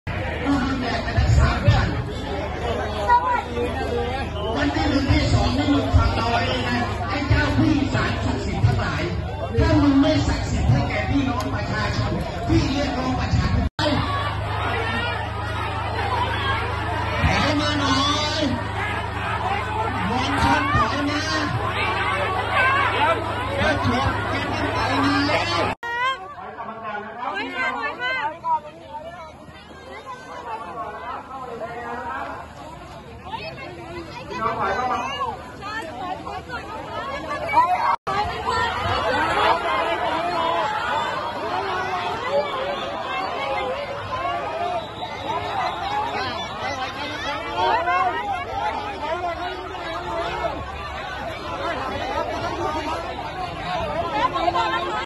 ผู้ชุมนุม ถ.สามเสน เข้าใกล้แนวรับ ตำรวจฉีดน้ำ ยิงแก๊สน้ำตาสกัด | 15.10 น. 17 พ.ย. 63
17 พ.ย. 63 เวลา 15.10 น. สถานการณ์ชุมนุมที่ ถ.สามเสน ด้านบางกระบือ การ์ดผู้ชุมนุมเข้าใกล้แนวรับของตำรวจ ทำให้ตำรวจฉีดน้ำและยิงแก๊สน้ำตาสกัด โดยการยิงแก๊สน้ำตาเกิดขึ้นตลอดทั้งวันบริเวณด้านนี้